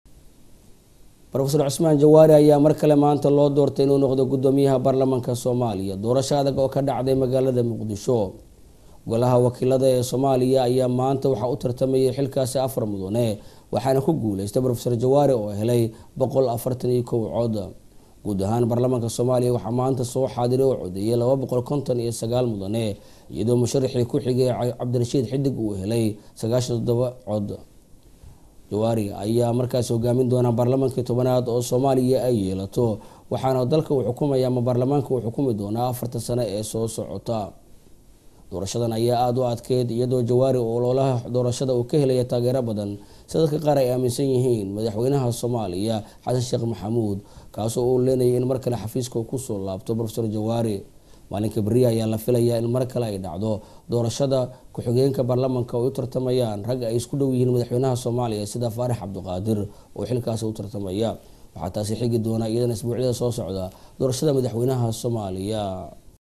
PROF MAXAMED CISMAAN JAWAARI OO MAR KALE DIB LOO DOORTAY
PROF MAXAMED CISMAAN JAWAARI OO MAR KALE DIB LOOGU DOORTAY GUDOOMIYAHA BAARLAMAANKA AQALKA HOOSE EE DFS